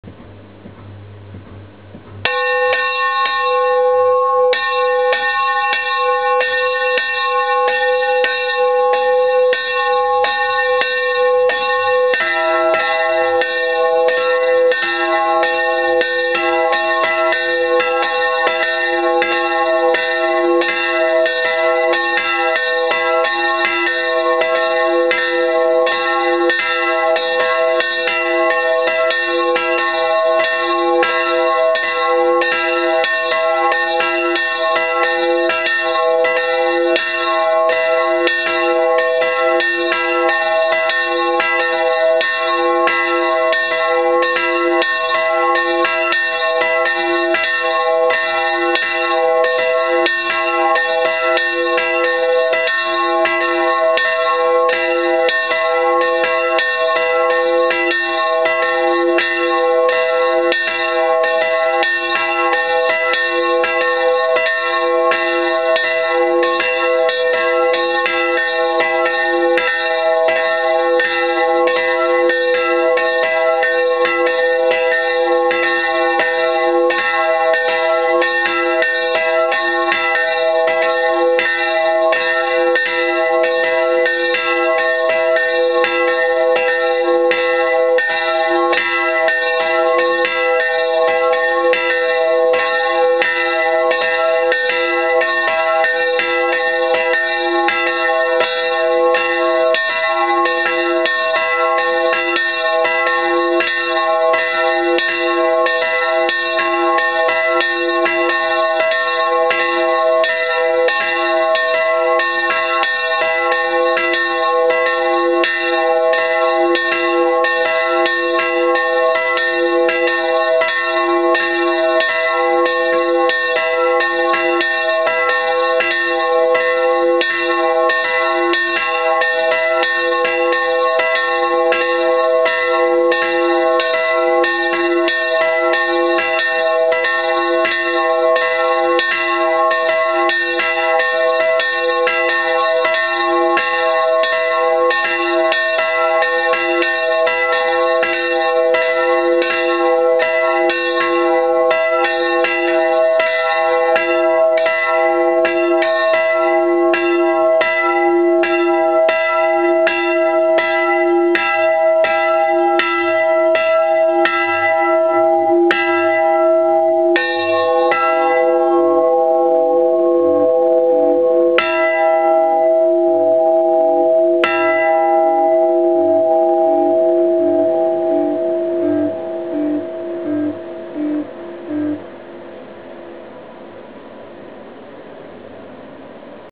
Geläute der Filialkirche Niederranna
Vollgeläute der Filialkirche von Niederranna an der Donau
(Pfarre Hofkirchen im Mühlkreis) in Oberösterreich.

Das Geläute besteht aus 2 Glocken die in den Tönen e´´ und a´´
erklingen. Die größere wurde 1948 in St Florian in Zinnarmer
Legierung gegossen und hing ursprünglich in Wesenufer.
Die kleinere wurde 1906 von Anton Gugg in Linz gegossen.

*Sondergeläute*

Der Pfarre ein herzliches Dankeschön für die Ermöglichung
der Turmbesteigung und das Sondergeläute.